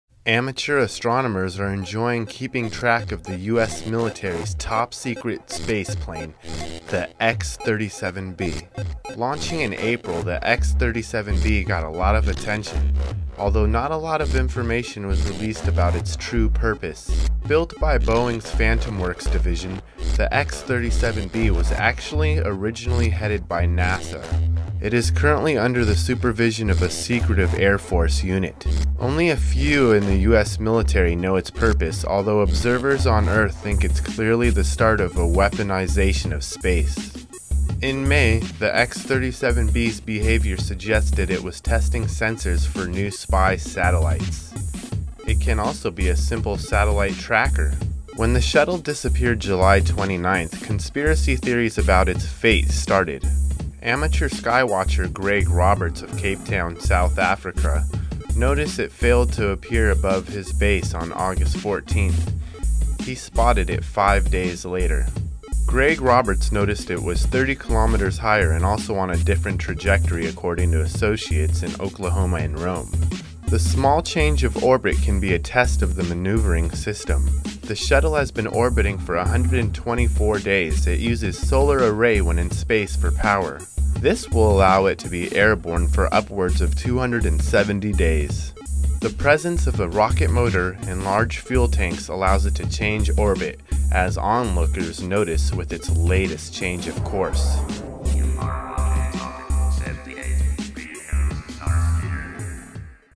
X-37B SHUTTLE DISAPPEARS!
AMATEUR astronomers are enjoying keeping track of the U.S. Militaries top secret space plane, the X-37B.

Launching in April, the X-37B got a lot of attention, although not a lot of information was released about it's true purpose.
Built by Boeing's Phantom Works division, the X-37B was actually originally headed by NASA.
It is currently under the supervision of a secretive Air Force unit.

Only a few in the US military know it's purpose, although observers on Earth think it's clearly the start
of the "weaponisation of space".

In May, the X-37B's behavior suggested it was testing sensors for new spy satellites.
It can also be a simple Satellite Tracker.

When the shuttle disappeared July 29th,
conspiracy theories about its fate started.

Amateur sky watcher Greg Roberts of Cape Town, South Africa, noticed it failed to appear
above his base on August 14. He spotted it five days later.

Greg Roberts noticed it was 30km higher and also on a different trajectory,
according to associates in Oklahoma and Rome.

The small change of orbit can be a test of the maneuvering system.

The shuttle has been orbiting for 124 days. It uses Solar array when in space for power,
this will allow it to be airborne for upwards of 270 days.

Although the presence of a rocket motor and large fuel tanks allows it to change orbit,
as onlookers noticed with it's latest change of course.